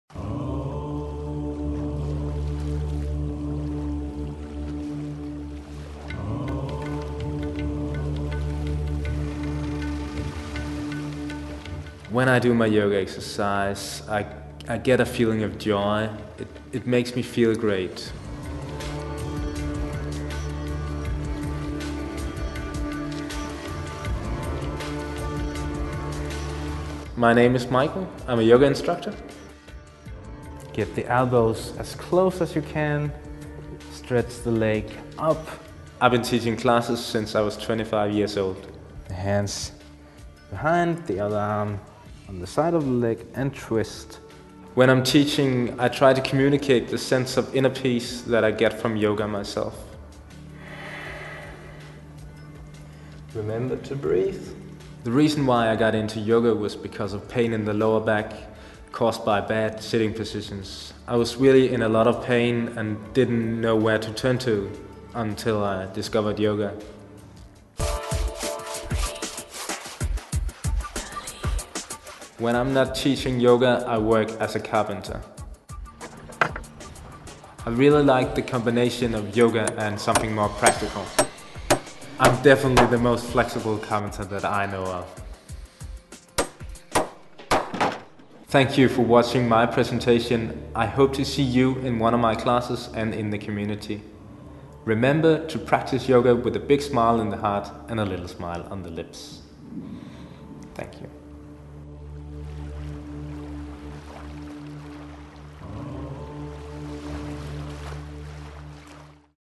Wexer instructor Michael Bjerrum- trainer presentation
Meet Michael Bjerrum.  Michael is one of the instructors at the online health club Wexer Michael teaches yoga classes which range from 5 to 45 minutes and vary in level of difficulty from beginner to advanced. You can participate in Michael´s classes for free by signing up at Wexer.com now.